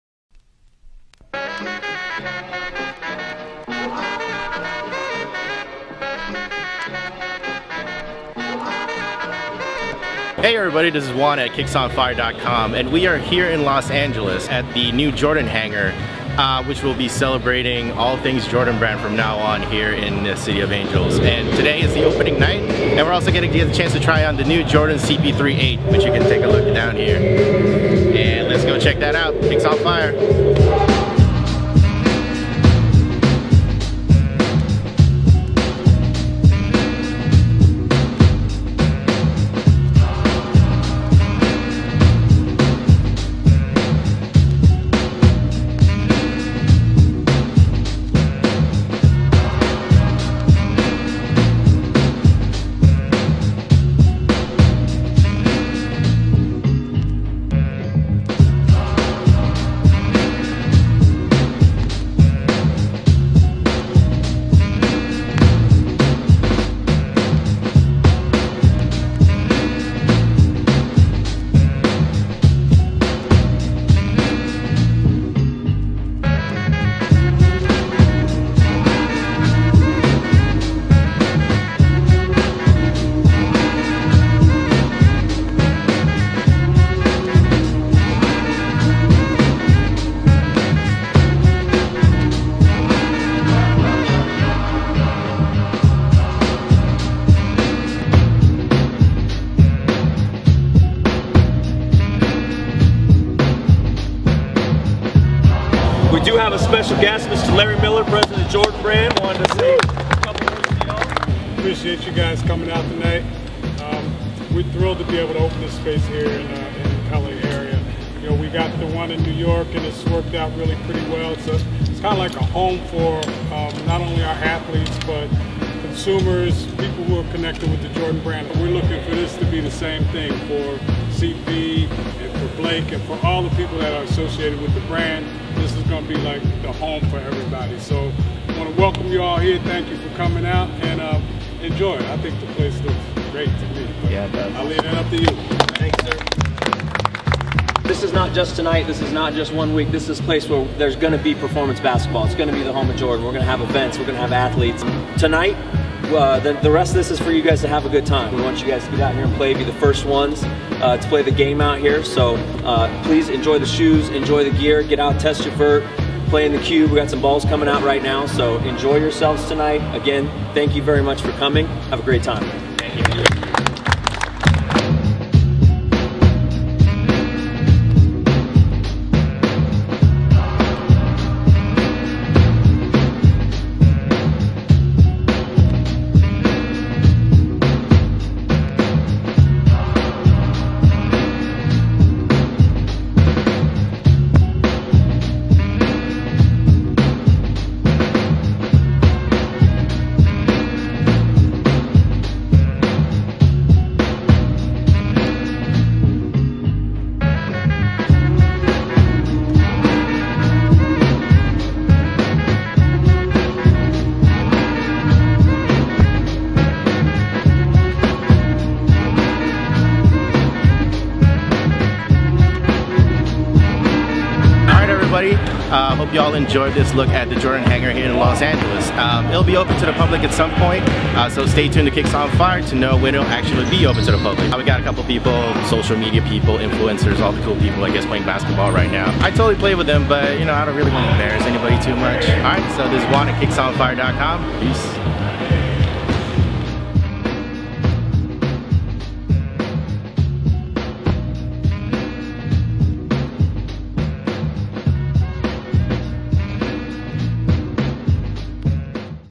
Kicks On Fire Visits The Jordan Hangar In Los Angeles
Jordan Brand has a new home in Los Angeles and we were there for a special preview night that featured a test run of the new Jordan CP3.VIII, the Air Jordan "History Of Flight" collection and a special appearance by Jordan Brand President Larry Miller.  Enjoy this special look at the Jordan Hangar.